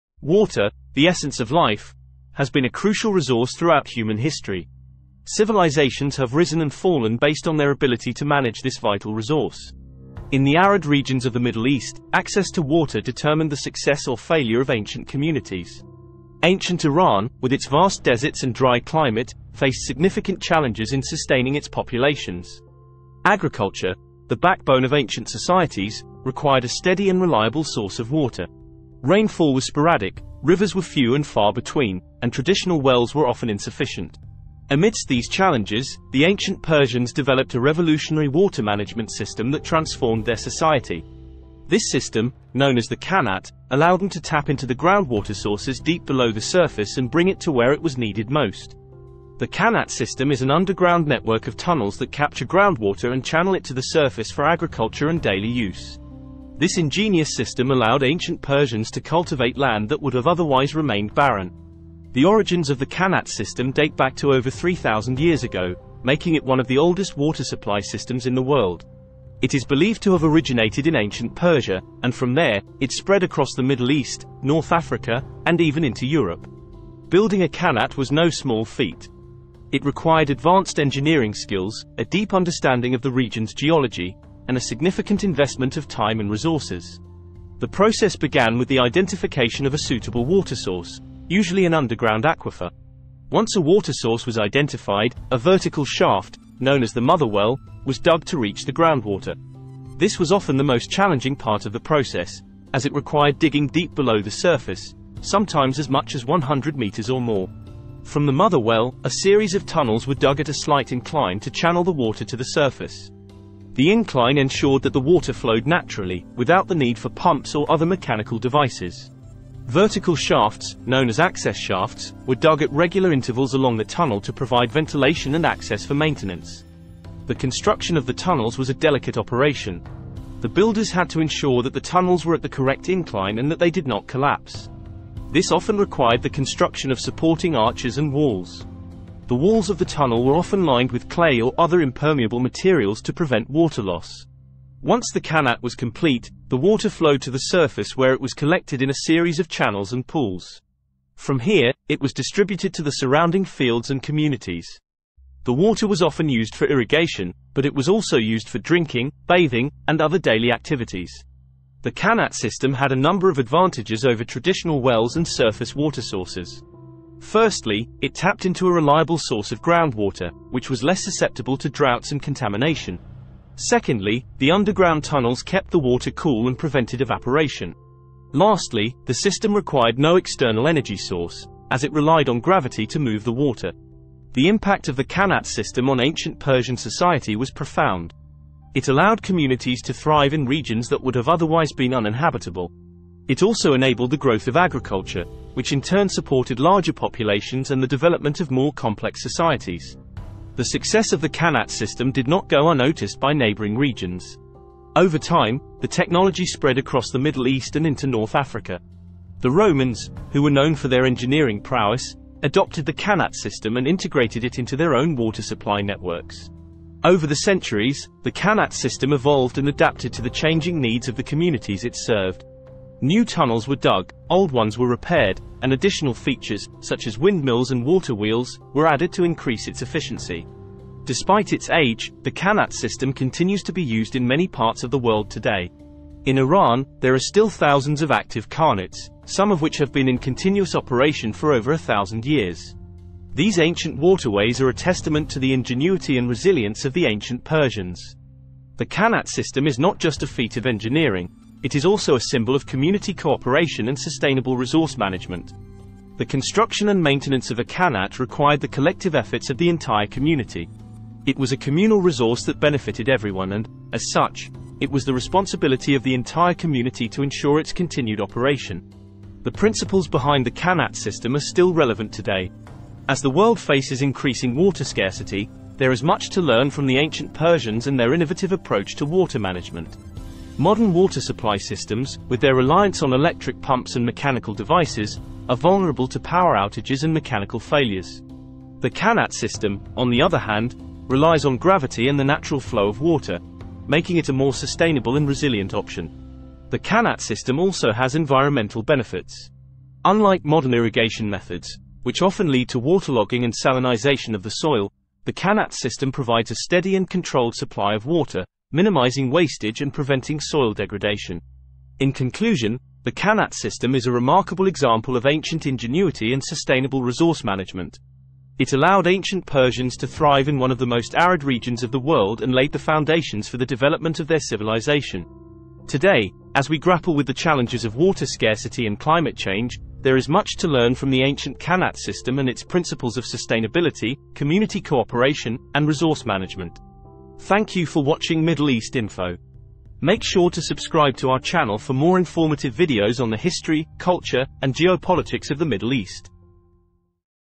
How Ancient Iran Built a Revolutionary Water System
Discover the engineering marvel of the ancient Persian Qanat system, a revolutionary underground network of tunnels that transformed water management and enabled civilizations to thrive in arid regions. Explore its impact, legacy, and relevance in today's water-scarce world.